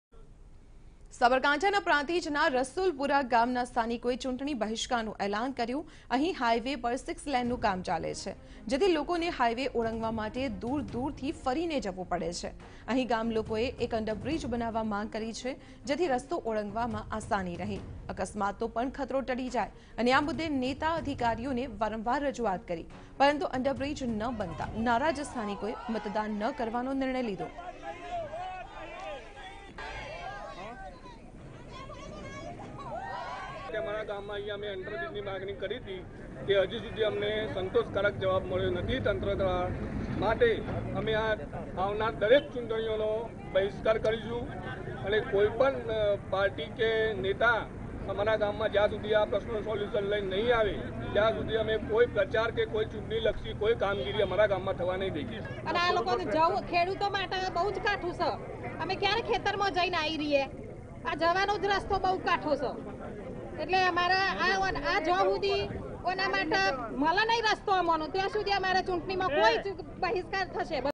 Residents boycott poll, demanding to complete national highway work | Sabarkantha | Tv9
A04